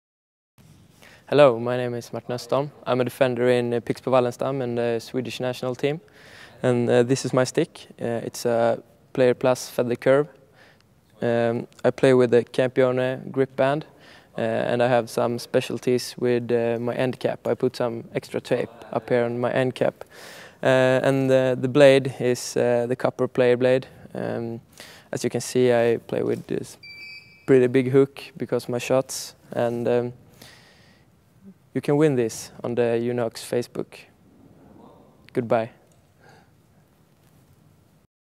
Martin Östholm about his Curve 2.0° stick!
Martin Östholm gives us a briefing about his choice for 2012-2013! You will have the chance of winning his stick between 529/10 until 4/11 so watch the video and participate! Good luck!